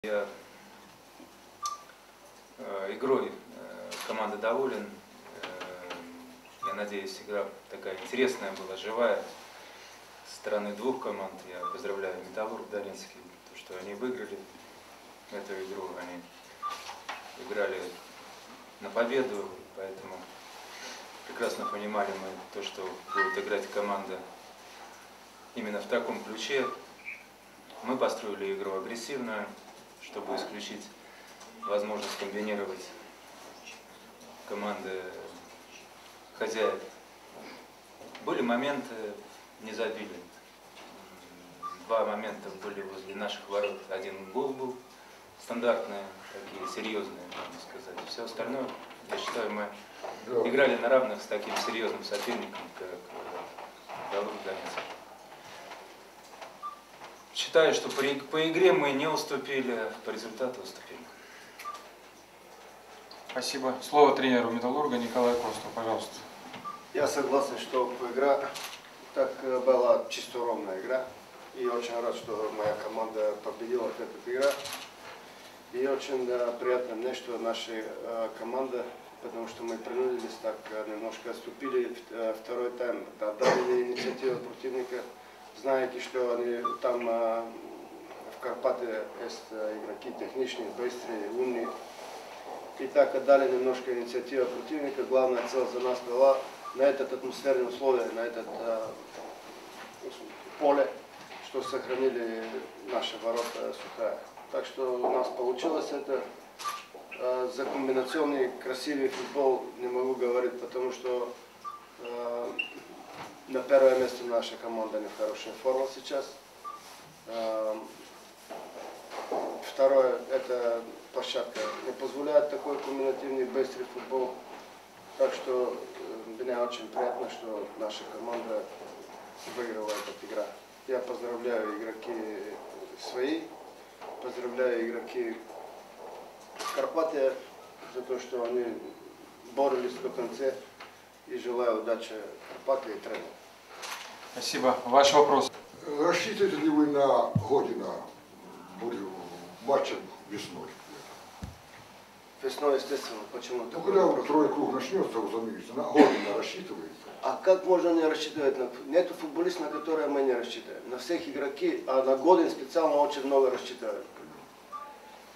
13-12-2009
Пресс-конференция Николая Костова и Олега Кононова